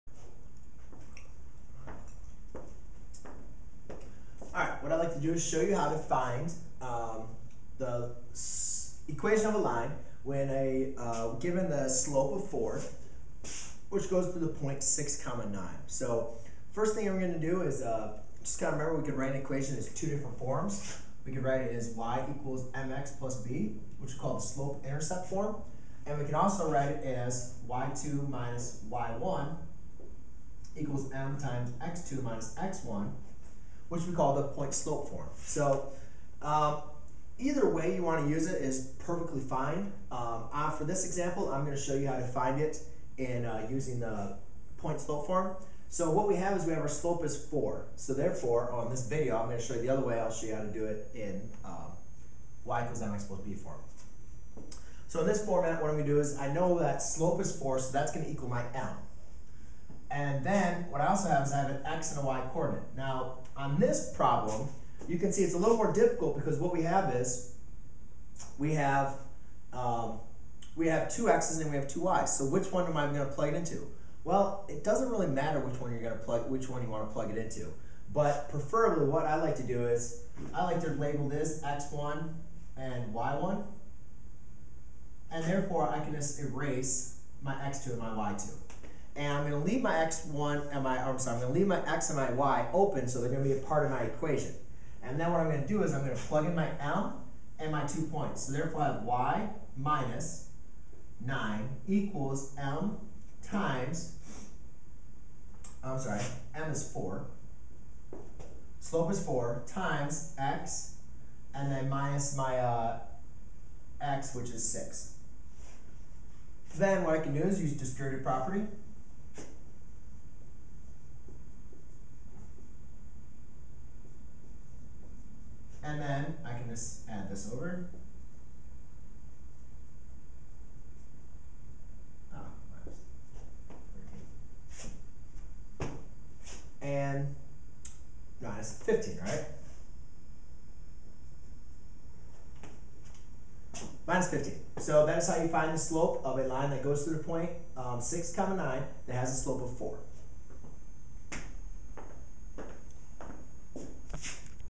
Finding the equation of a line using point slope form given a point and slope
👉 Learn how to write the equation of a line in a point-slope form. The equation of a line is such that its highest exponent on its variable(s) is 1. (i.e. there are no exponents in its variable(s)). There are various forms which we can write the equation of a line: the point-slope form, the slope-intercept form, the standard form, etc.

The point slope form of the equation of a line is given by y - y1 = m(x - x1). where (x1, y1) is a point on the line and m is the slope of the line.